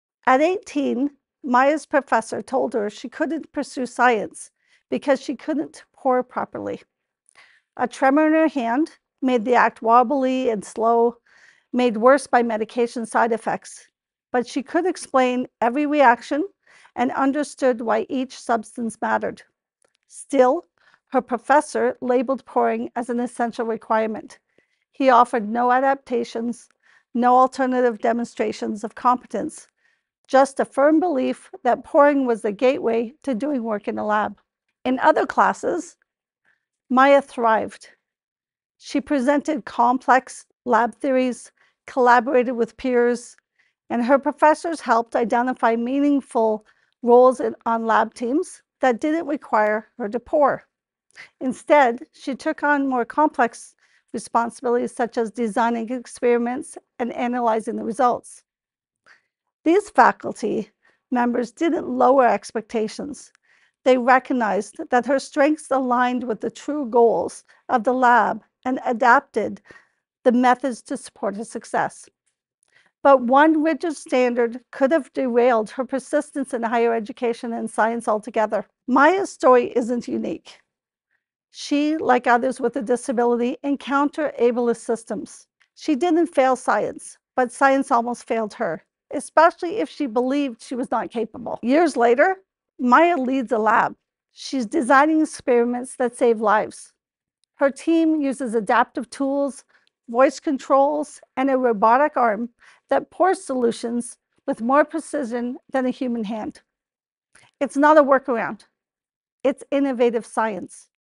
How a young scientist overcame systemic barriers 💫
For every disabled student who builds a future in STEM, many others are pushed out before they ever get the chance. Not because they lack talent, but because the system measures conformity over innovation and only offers one way to demonstrate mastery. When essential requirements go unexamined, covert ableism goes unquestioned, and we lose brilliant minds who could transform science.

[VD: Tina Doyle sits in a black armchair on a dark stage. She wears glasses and a textured red shawl over a black outfit. Soft stage lighting highlights her as she speaks.]